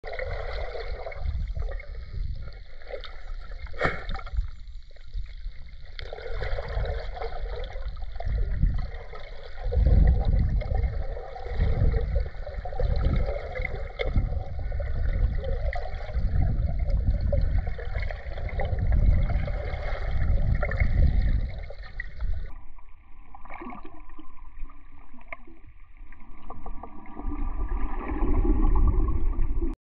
Swimming with a Bowfin
A underwater swim with a bowfin. The bowfin (Amia calva) is also called the grindle, blackfish, or freshwater dogfish. The bowfin is mottled green and brown and has a long dorsal fin and strong conical teeth.